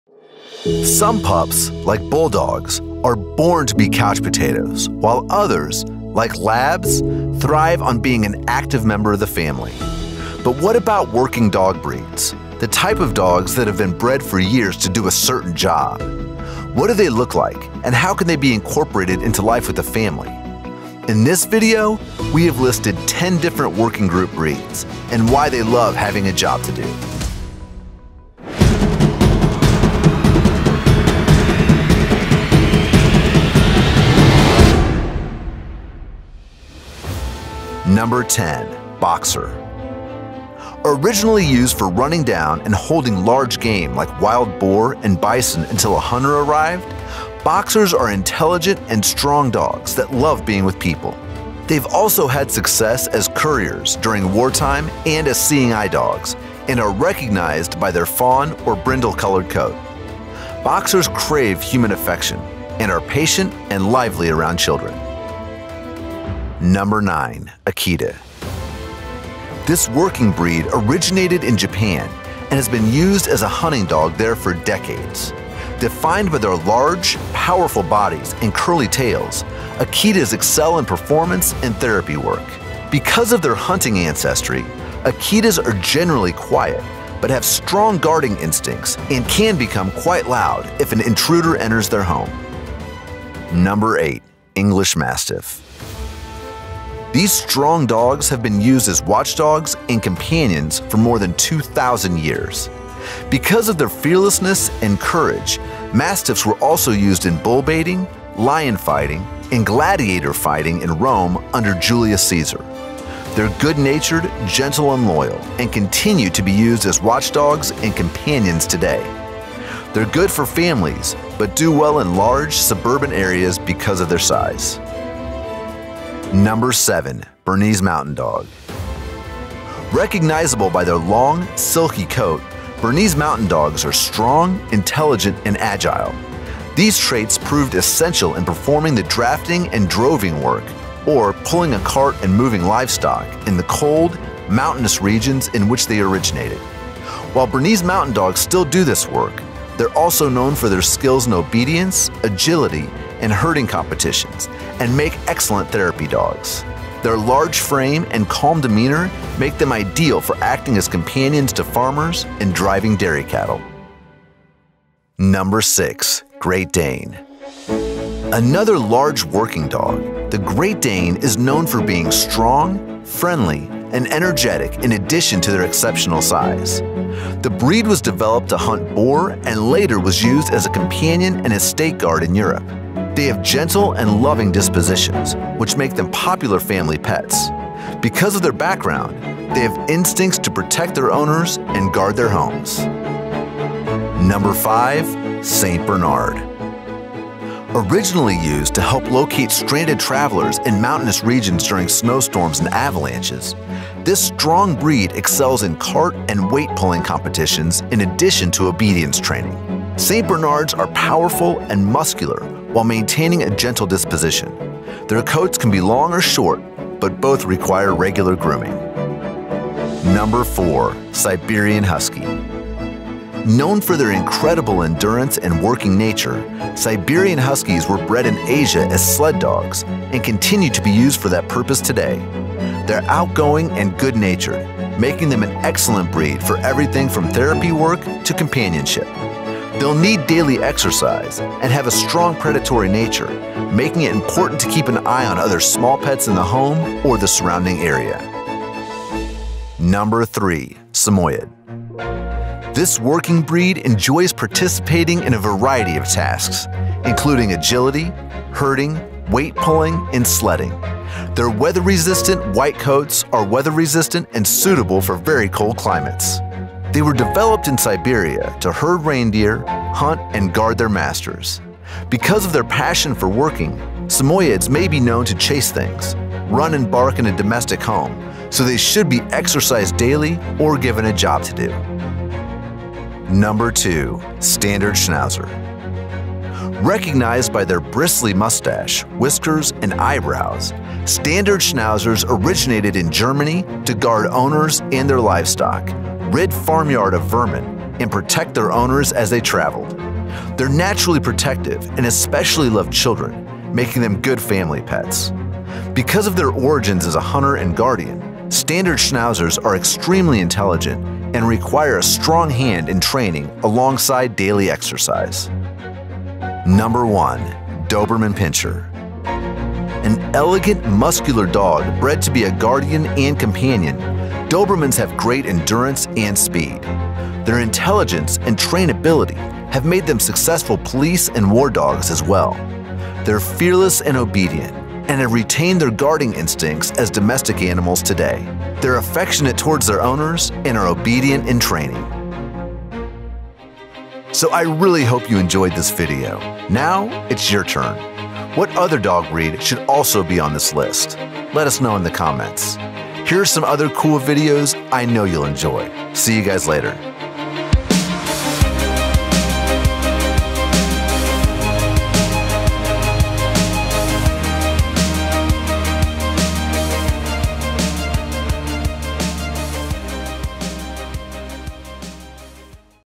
10 Dog Breeds That Love to Work
Some pups, like bulldogs, are born to be couch potatoes while others, like labs, thrive on being an active member of the family.
But what about working dog breeds — the type of dogs that have been bred for years to do a certain job?
What do they look like and how can they be incorporated into life with a family?
In this video we have listed 10 different working group breeds, and why they love having a job to do.